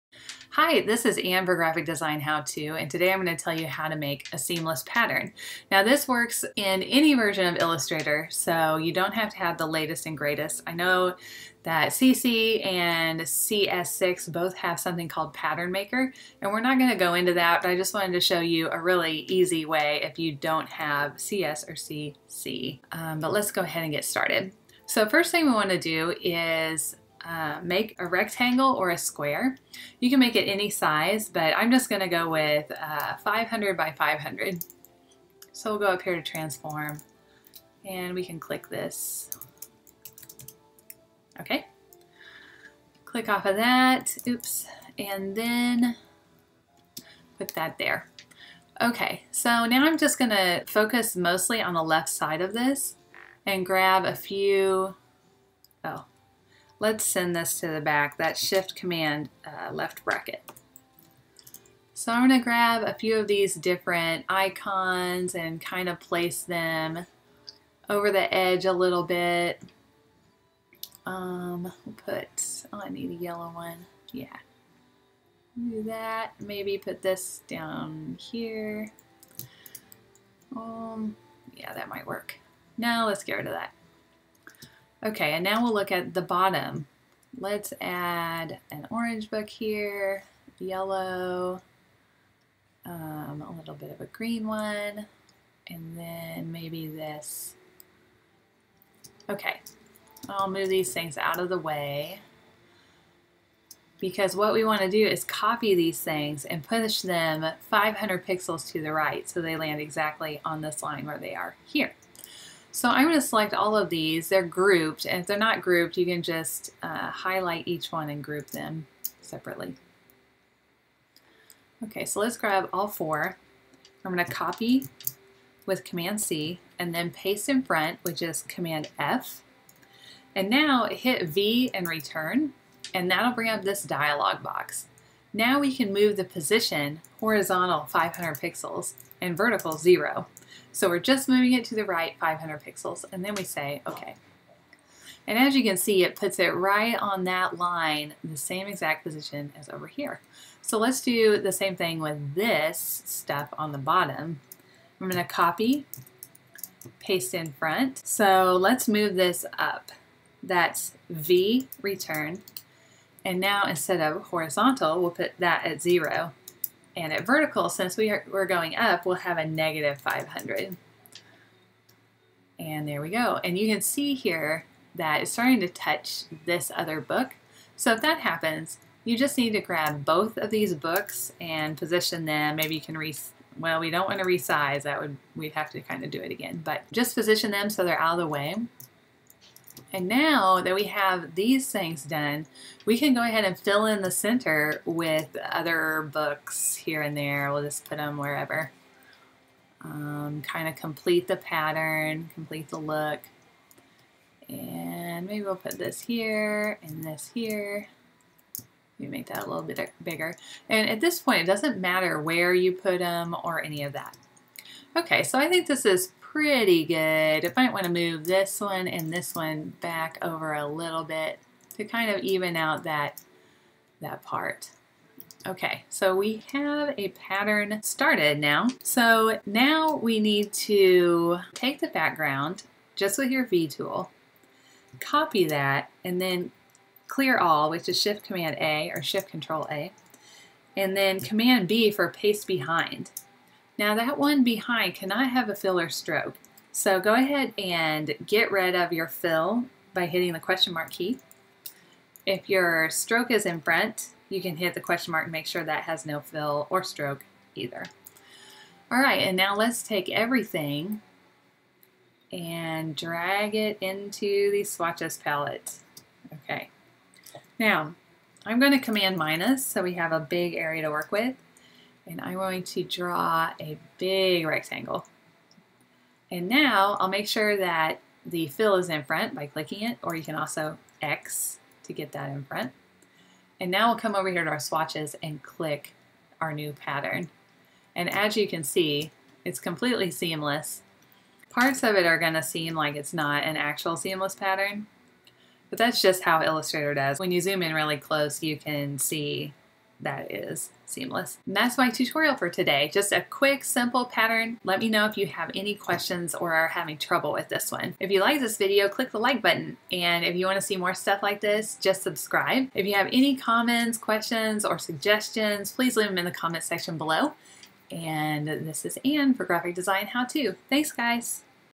How to make a Seamless Pattern in Adobe Illustrator - 5 Minute Tutorial - Graphic Design How to
Making a seamless pattern is super easy with Adobe Illustrator CS6 and CC, but what if you have an older version, or want more control over your pattern? Check out a super easy way to make patterns!